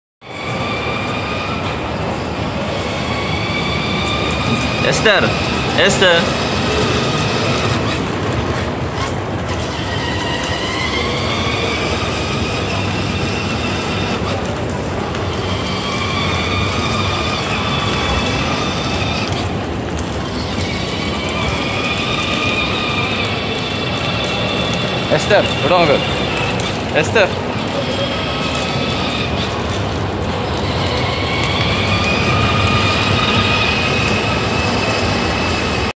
Esther on wheels
Esther in a remote car at Gold souk